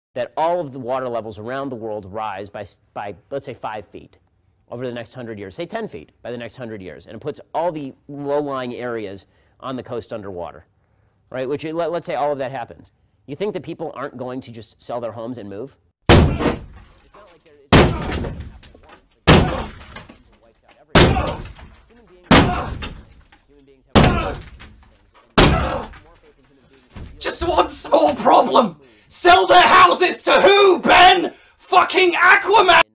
SELL THEIR HOUSES TO WHO, BEN?
, so the video can be found and shared more easily. Follow the creator for similar stuff, not me.
Scotty you can order me to take this down if you want.
A clever li'l edit of hbomberguy's reaction to Ben Shapiro's sheer logic.